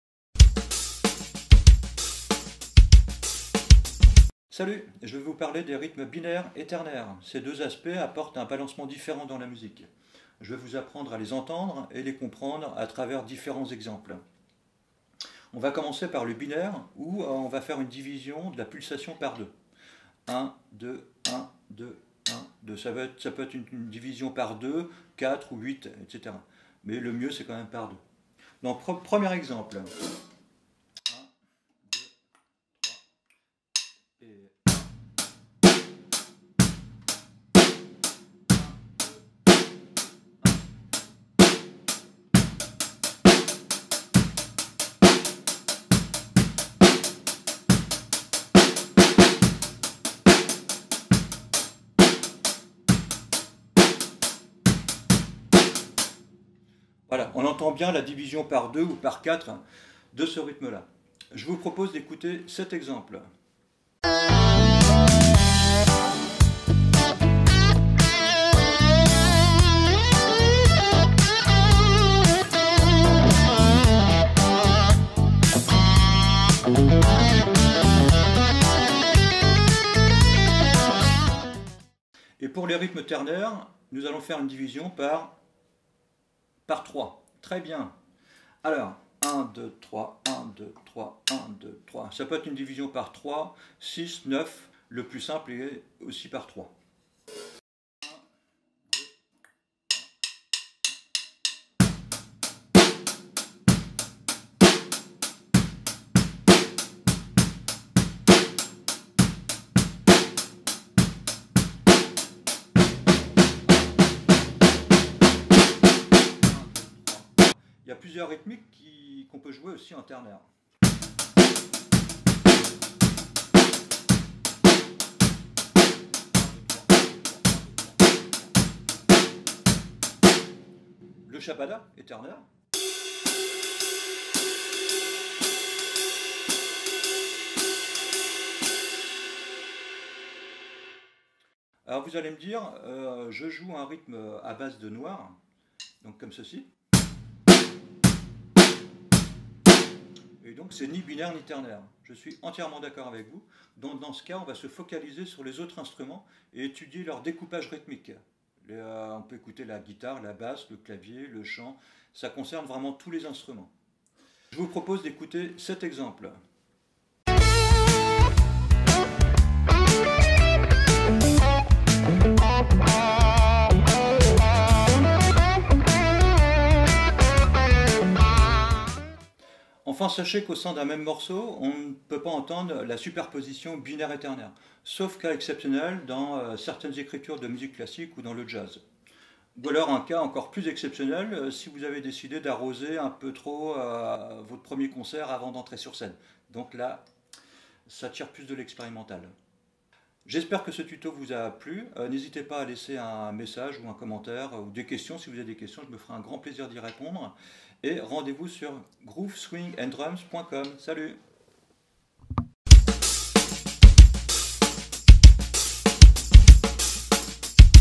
Les rythmes BINAIRES/TERNAIRES à la batterie.
Je vous présente une approche (raccourcie) de ces deux aspects rythmiques.
Le BINAIRE et le TERNAIRE apportent un balancement différent à la musique.
Lorsque vous écoutez vos titres préférés, entrainez vous à savoir de quel côté penche le rythme :)